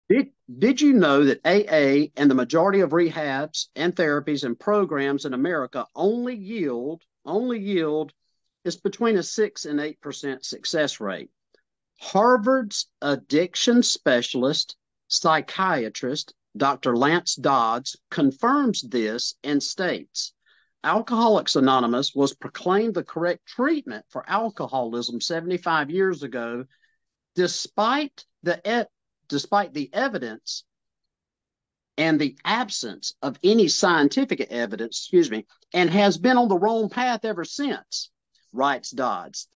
The Truth About Inpatient Facilities
Discover the Shocking Truth About AA and Rehab Success Rates! 🤯 Did you know that the majority of rehabs and therapies in America only have a 6-8% success rate? 😱 Harvard's addiction specialist, Dr. Lance Dodds, confirms this alarming statistic and reveals the flawed history of Alcoholics Anonymous (AA). Despite being proclaimed as the correct treatment for alcoholism 75 years ago, AA lacks scientific evidence and has been on the wrong path ever since. 🚫 Don't miss this eye-opening discussion on addiction treatment!